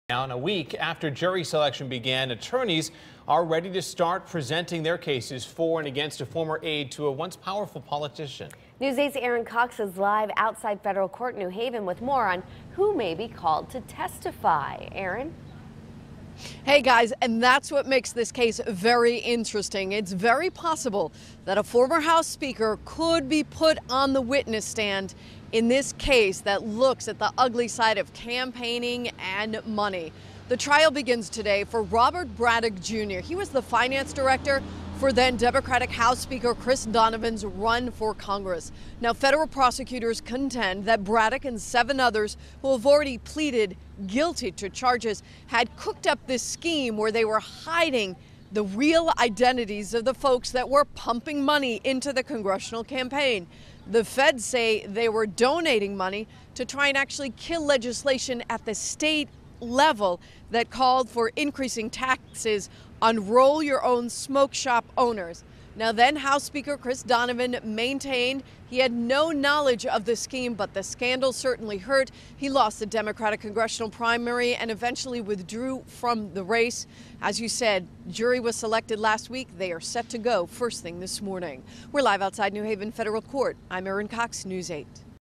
Testimony begins in Braddock trial
(WTNH) -- A case that could see former Connecticut House Speaker Chris Donovan called to the witness stand is on the docket in federal court today.